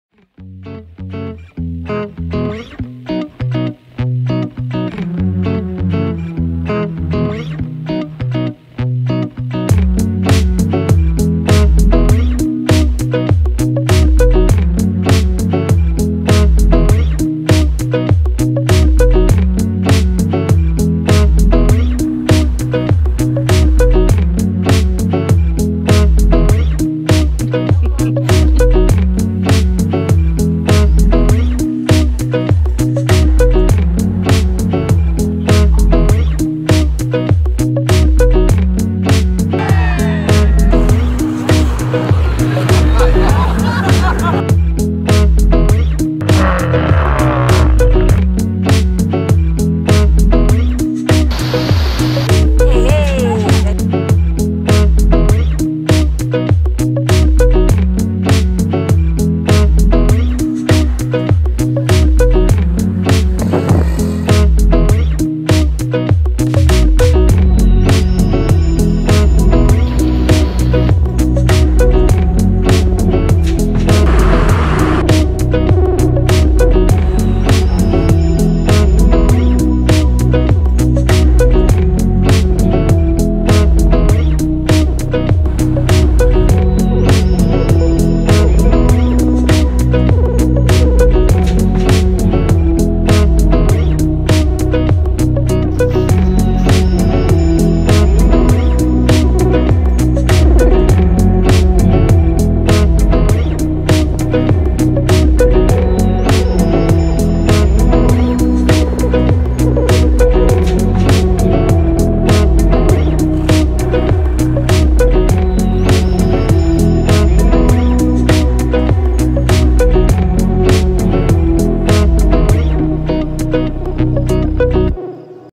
Africa 4x4 Tour
Africa - Namibia, Botswana & Zimbabwe Self Drive Tour offered by Overland Journeys in cooperation with a well established Australian company. Namibia and Botswana are two dramatically different countries and you can combine and see these differences in one amazing tour. We also take an overnight side trip to visit the mighty Victoria Falls in Zimbabwe. You will be provided with 4x4 vehicle for self-drive experience.